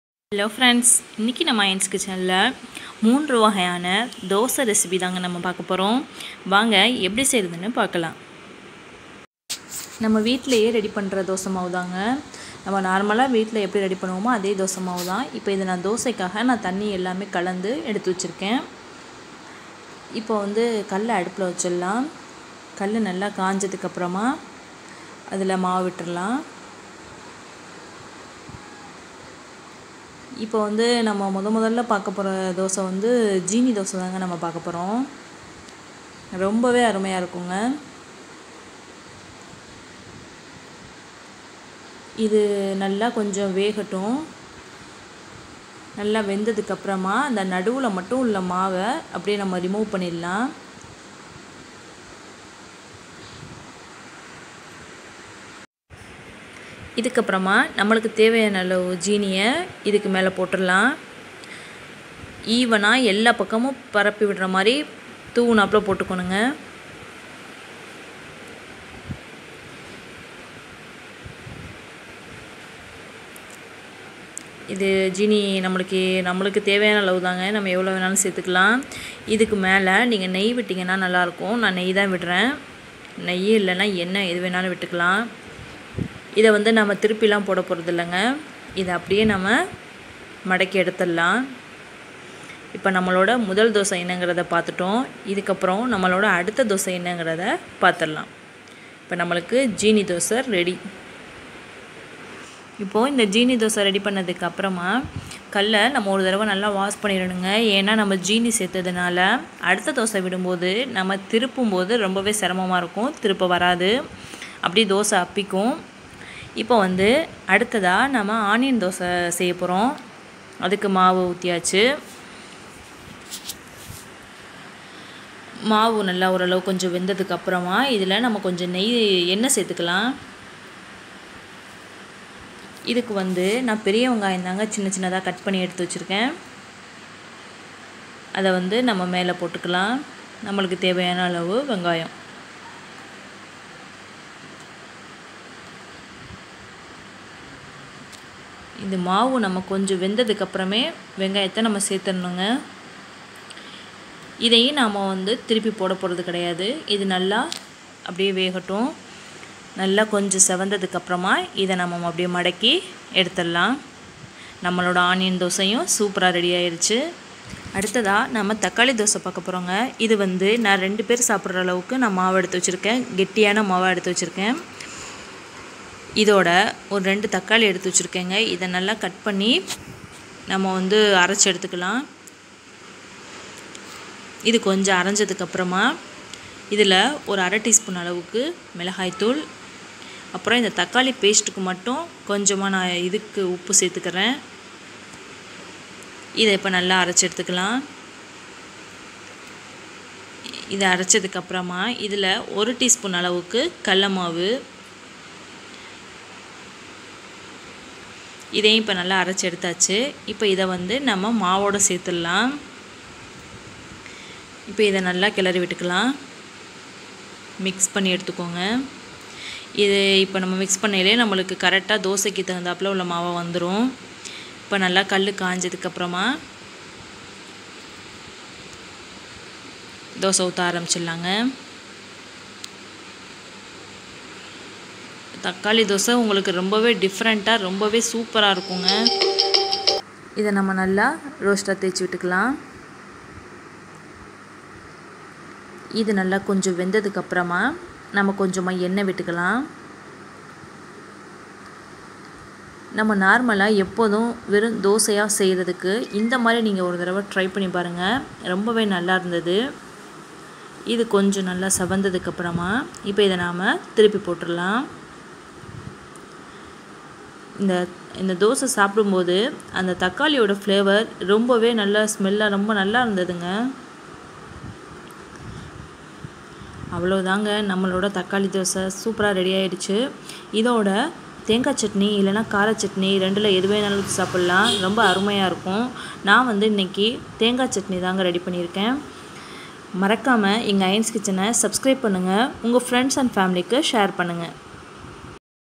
Dosa Varieties at Home(3 Types of Dosa)
In this video we will going to see about how to make dosa varieties at home.Three 3 diffierent types of dosa we can see in this video.it is easy and simple to make let see how to make sugar dosa ,onion dosa and tomato dosa.